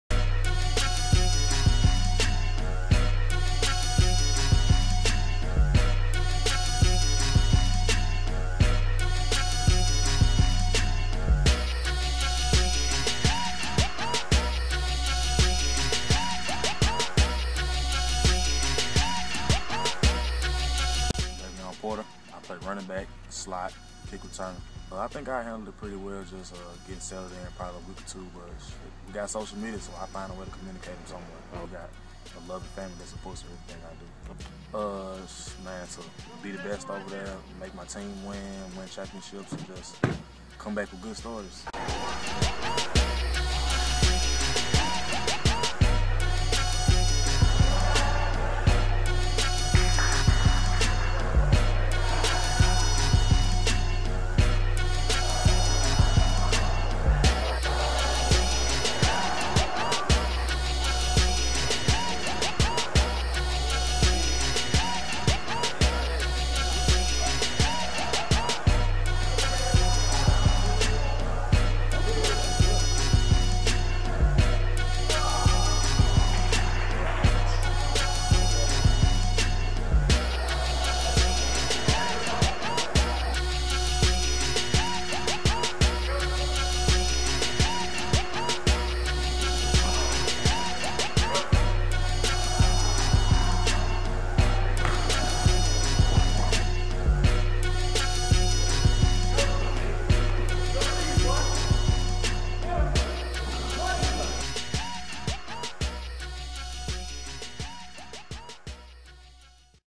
Daveonn Porter Dallas Pro Hunt Combine
Daveon Porter
5'8" 180lbs (RB)

College: Texas Southern University

PRO HUNT RATING: 3.5/5*
*Player injury prevented full evaluation

*SPEED SPEED SPEED!